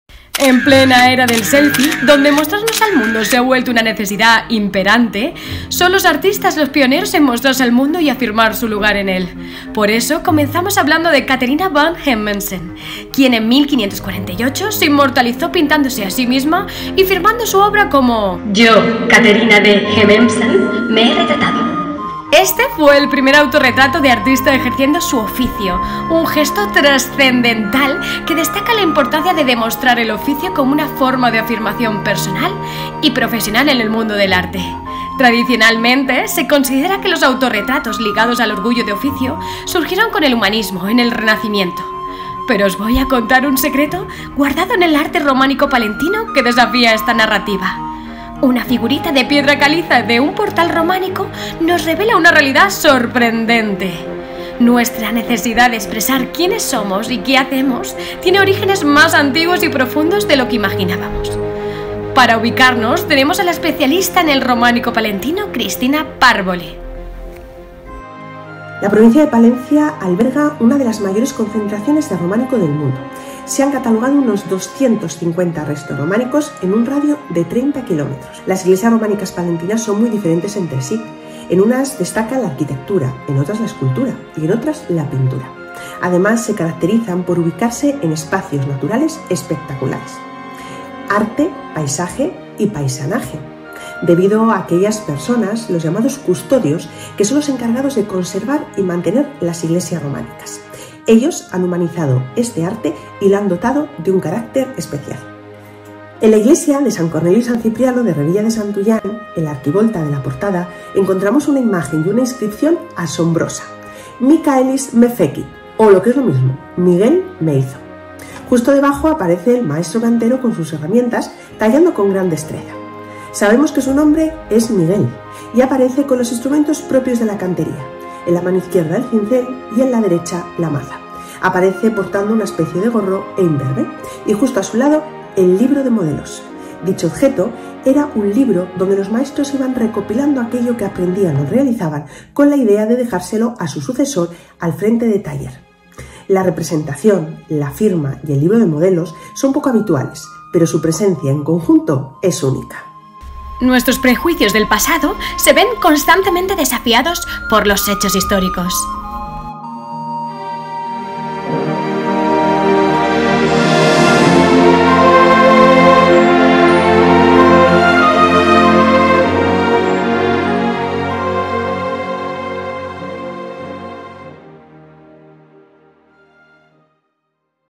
Desafiando Prejuicios: El Misterio del Autorretrato en el Románico Palentino.
Prepárense para desafiar prejuicios y sumergirse en el misterioso mundo del románico palentino. En colaboración con  @lahuellaromanica3174   exploraremos un tema poco conocido en el arte románico: el autorretrato. Con la valiosa guía de la especialista en románico palentino, Cristina Párbole, nos adentraremos en una región de España donde el arte y el paisaje se fusionan de manera única.

Descubriremos cómo el paisanaje desempeña un papel vital en la preservación del patrimonio, siendo los propios habitantes quienes protegen las pequeñas iglesias diseminadas en apenas 30 kilómetros. Esta conexión entre arte y comunidad nos revelará la importancia de cuidar y apreciar nuestro legado cultural.